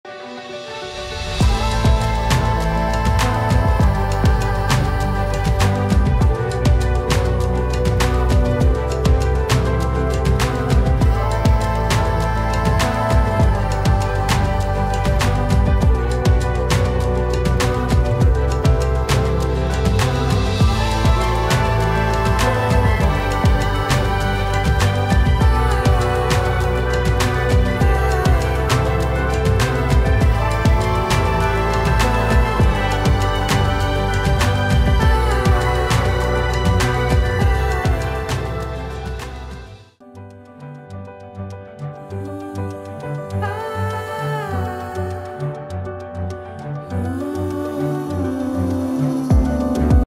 Fly over Northwest Montana's beautiful Flathead Valley
See the Swan River, Flathead Lake, Village of Bigfork, and more of Montana's treasures in this quick trip over the Flathead Valley. Footage courtesy of Northwest Montana Partners, a team of high producing real estate agents with National Parks Realty Forbes Global Properties. Love where you live!